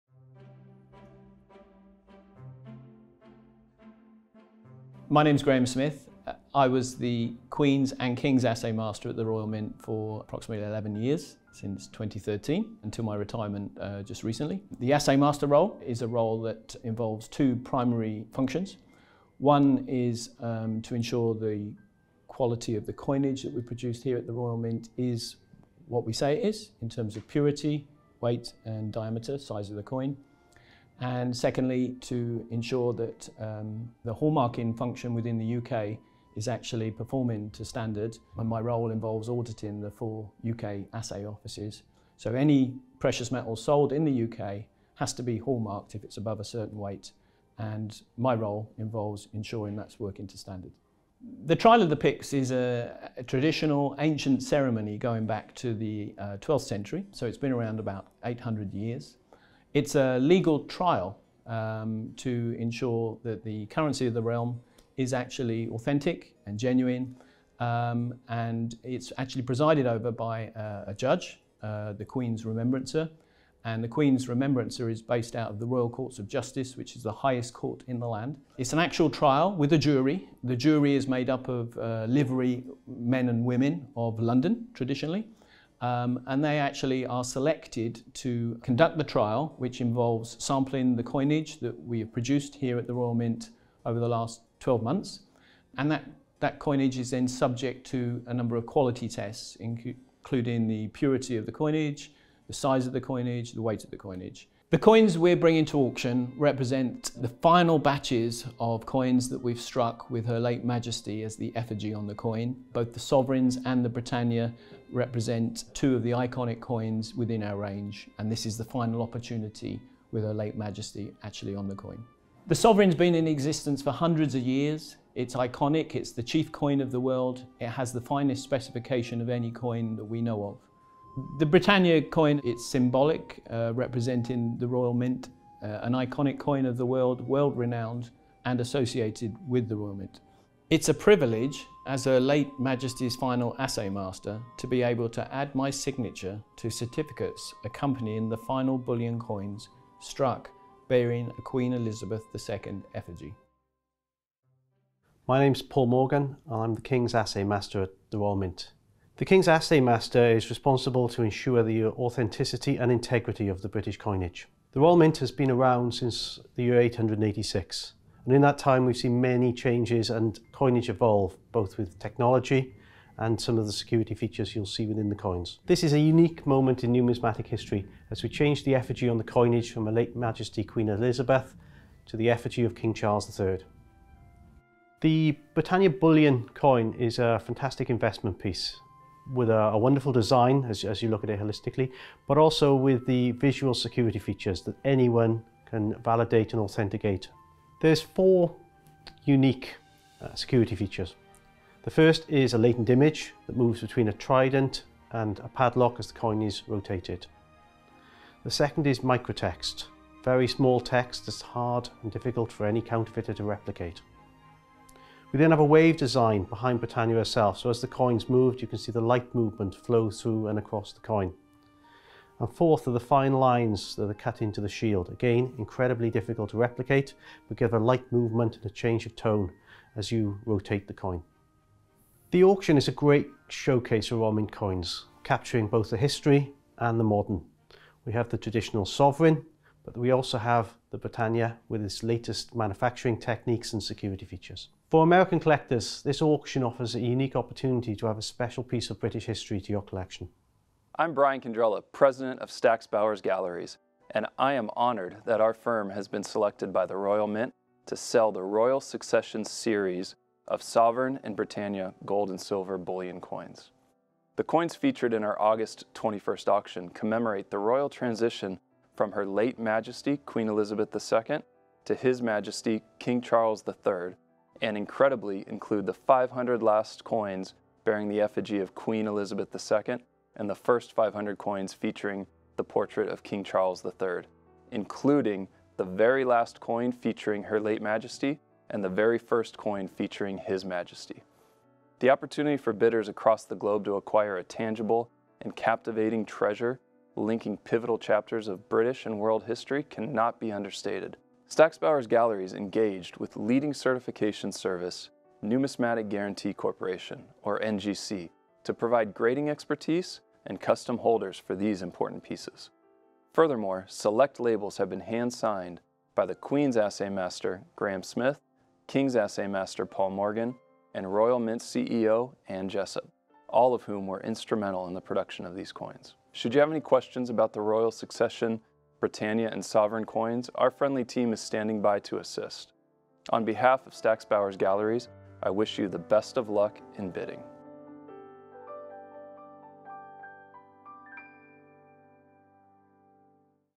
The British Royal Mint Selects Stack’s Bowers to Sell Royal Succession Britannia and Sovereign Coins
Christine Karstedt, Executive Vice President

The British Royal Mint Selects Stack’s Bowers Galleries to Sell Historic Royal Succession Britannia and Sovereign Coins

Stack’s Bowers Galleries is honored to announce that they have been selected by the British Royal Mint to auction a selection of special Britannia and Sovereign gold and silver bullion coins commemorating the royal transition from Queen Elizabeth II to King Charles III. Entitled the Royal Succession coins series, these historic pieces represent the very last coins struck under the reign of Queen Elizabeth II and the very first coins produced in the name of King Charles III for each series. They serve as tangible and captivating treasures linking two pivotal chapters of British and world history.

Stack’s Bowers Galleries is honored to have been selected by the British Royal Mint to auction the Royal Succession Sovereign and Britannia bullion coins and is excited to present them to the collecting community in the August 2023 Global Showcase Auction. For more information about these historic Royal Succession coins or to register for bidding in the auction, please contact Stack's Bowers Galleries today at or .